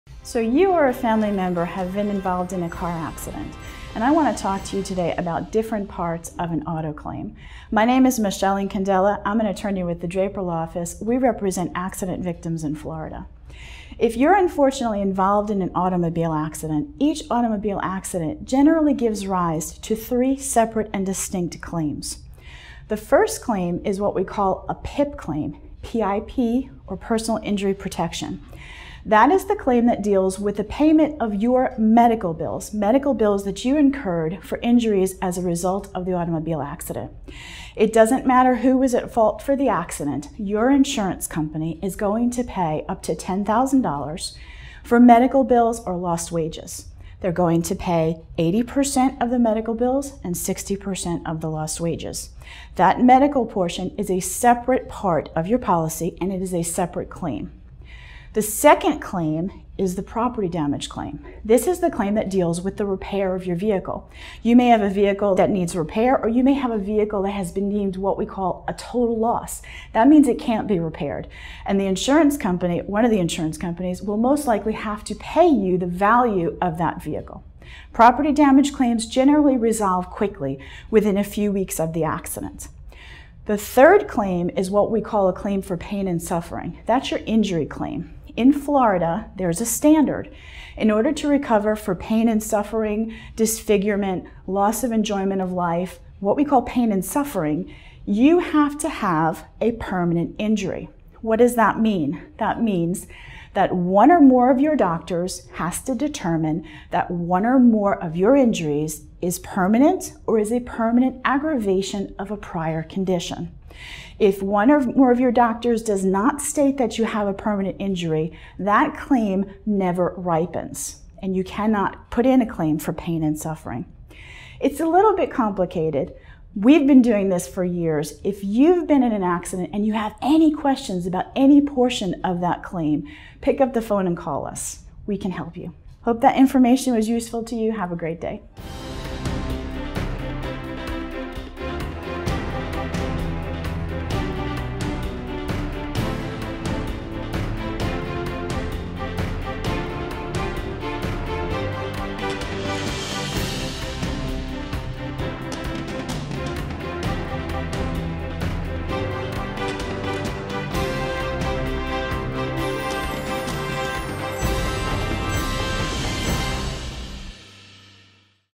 Explaining The Three Claims Arising From an Auto Accident – FL Injury Attorney Michelle Incandela
Listen as Michelle Incandela discusses the three distinct claims that arise following an automobile accident.

The first claim is for personal injury protection, which is often referred to as PIP.  This claim involves the payment of your medical bills resulting from the accident.  Regardless of fault, your insurance company will pay eighty percent of the medical bills and sixty percent of the lost wages, up to ten thousand dollars.

The second claim is for property damage, and it involves repairing your vehicle or compensating you for it, if it has been deemed a total loss.  These claims typically resolve within a few weeks of the accident.  Finally, the third claim is in regards to pain and suffering.  If one or more doctors determined that you have a permanent injury, you may be entitled to compensation for these injuries.  Watch the video to learn more.

I welcome your call.  where you can view more videos and media content from our law firm.